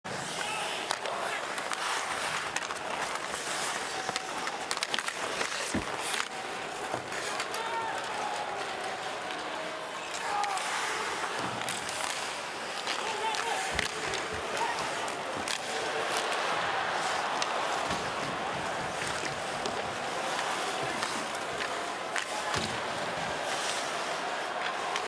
McDavid Hits Sorensen - Alternate Angle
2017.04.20
Sharks v. Oilers Game 5